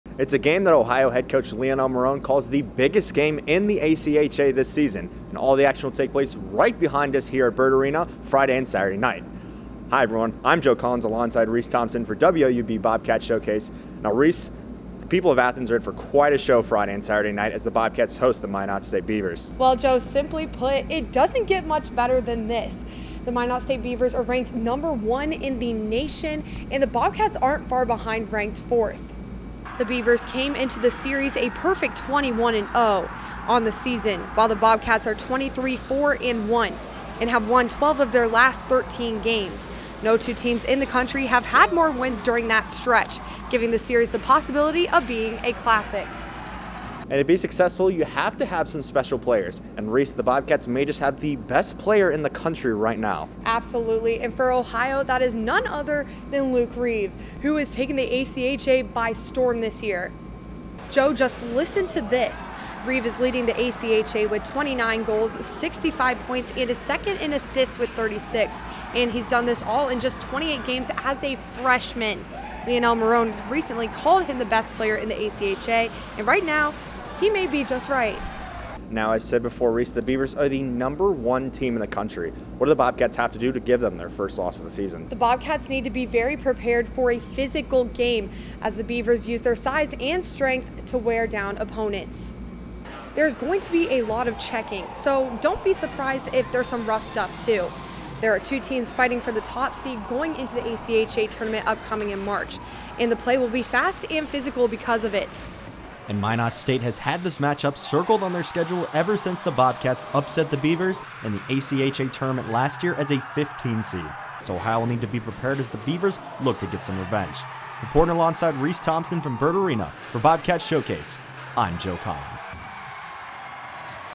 Ohio hockey faces the number one team Minot State for a weekend series
Ohio hockey faces its biggest test of the season this weekend as the number one team Minot State comes to town

The Bobcats must come out fast and physical in order to compete with the Beavers

🎥: Reese Thompson & Joe Collins